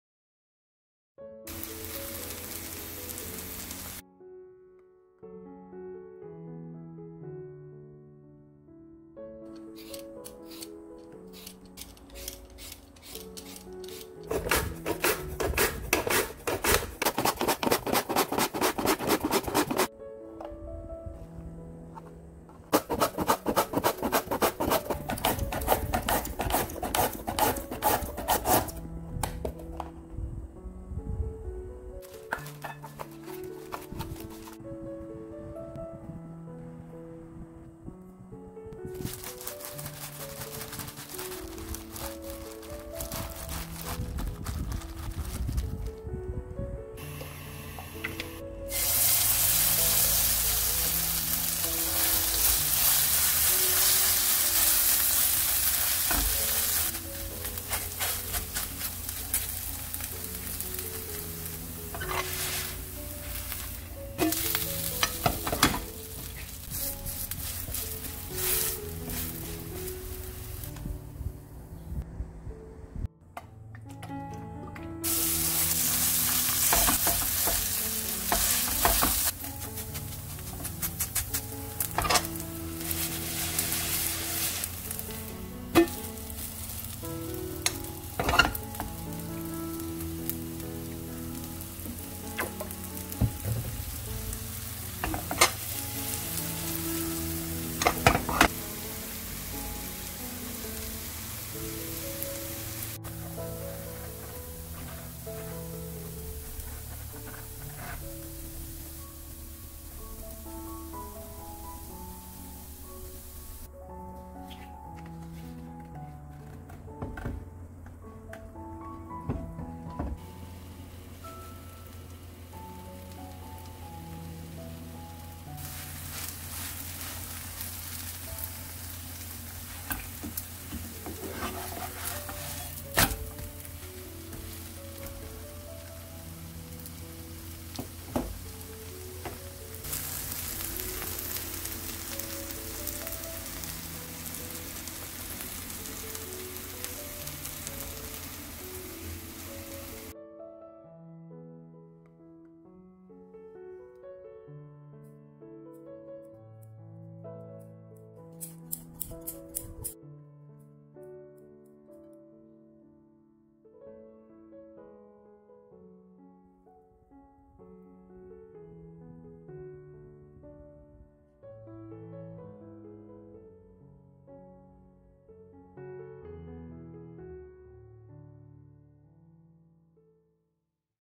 🥔감자전 맛있게 하는 방법 치즈품은 감자전 만들기
감자전 맛있게 하는 방법
치즈품은 감자전 만들기

재료
감자 3개
당근조금
치즈 70g
소금 1/3스푼
전분가루 3스푼
식용유